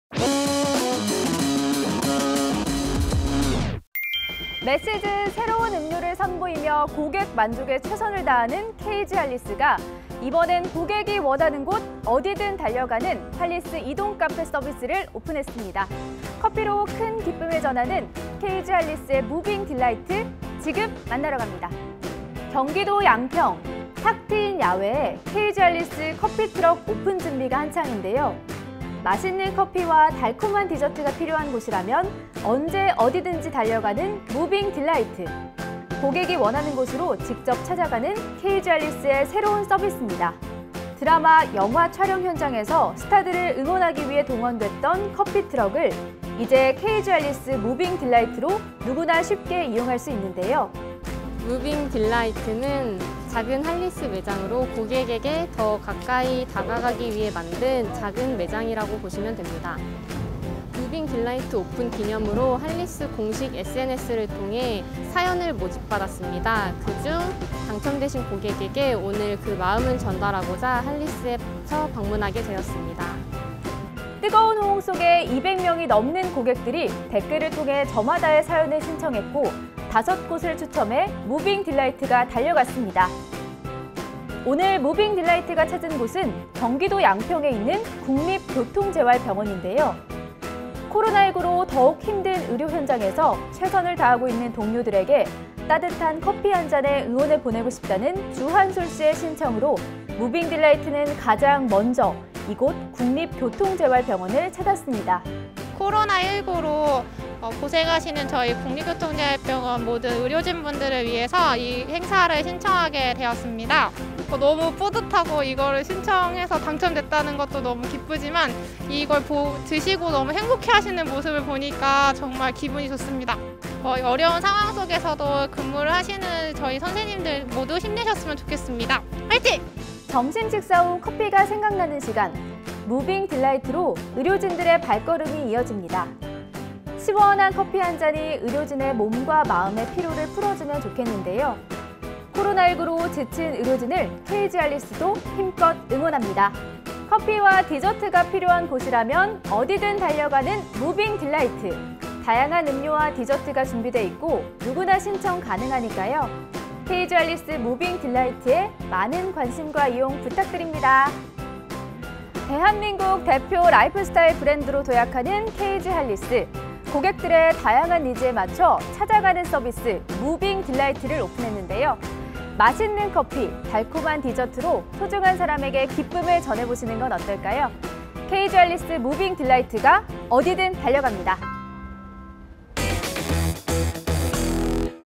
언제, 어디든 고객이 원하는 곳으로! KG 할리스 "무빙 딜라이트"
"무빙 딜라이트" 오픈을 기념하기 위해
할리스를 사랑하는 고객들의 오피스에 커피와 디저트를 무료로 보내드리는 이벤트를 진행했는데요.

뜨거운 호응 속에 200여 명이 넘는 많은 고객들이 저마다의 사연으로 무빙 딜라이트를 신청했습니다.
이 많은 신청자 중 무빙 딜라이트가 가장 먼저 달려간 곳은?

바로, 코로나 19로 힘든 의료 현장에 따뜻한 커피와 달달한 디저트 그리고 힐링을 싣고 열심히 달려갔습니다.
그 생생한 현장을 KG뉴스에서 확인하실 수 있는데요.

지역사회와 함께 성장하기 위해 다양한 활동을 펼치며 ESG경영에 앞장서고 있는 KG할리스!
그 현장, 다 같이 확인해 보실까요?